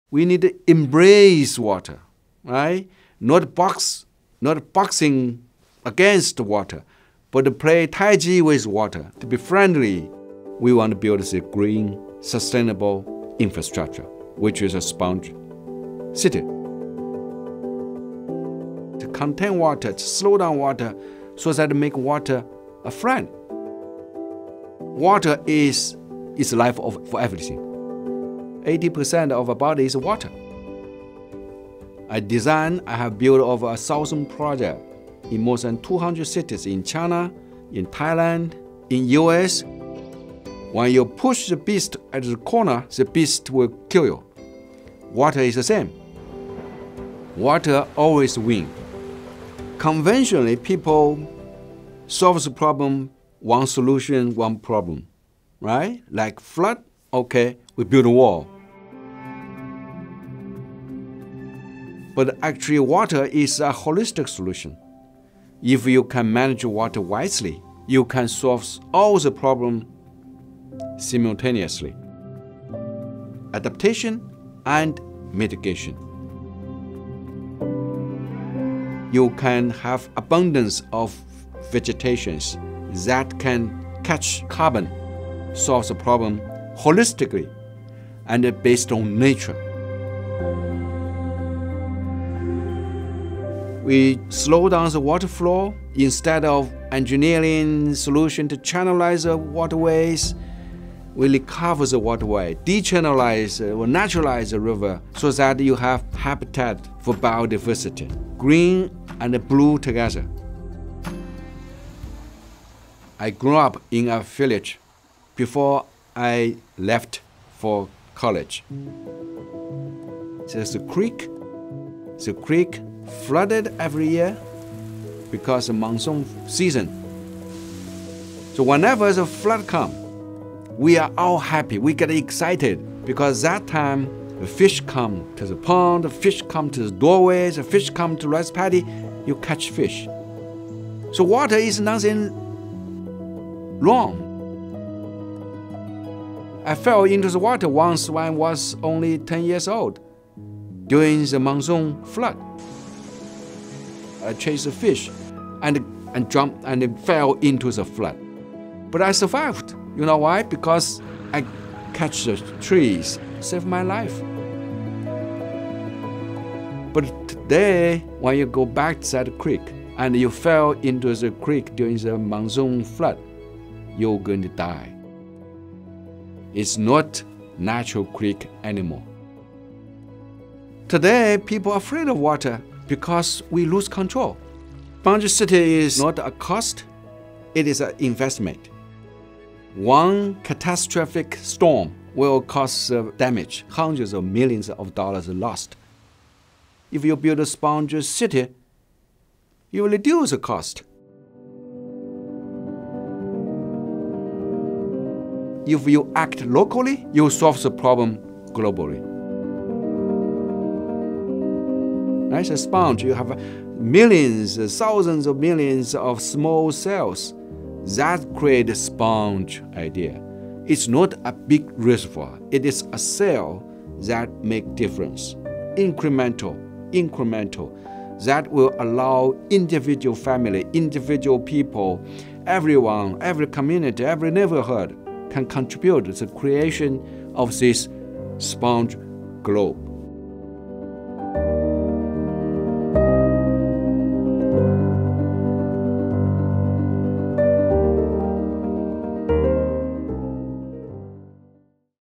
Flood-Proof Cities Concept – Professor Kongjian Yu's Sponge Cities
How can we work with water, rather than against it, to prevent flooding? Professor Kongjian Yu of Peking University, a pioneer of the “sponge city” concept, dives into holistic, nature-based solutions that can be used in water management. He describes how water is like a beast when it’s backed into a corner: it always wins. But if we can adapt and mitigate, we can work with water to solve the problem both locally and globally. Drawing on the story of how he almost drowned as a child during monsoon season, he advocates for naturalized waterways, which can capture carbon as well as act as a sponge to absorb excess water. He envisions a world where everyone can participate in the sponge cities concept!

Thanks to the James and Cathleen D. Stone Foundation for organizing Professor Yu's visit and for supporting this collaboration with the Stone Living Lab.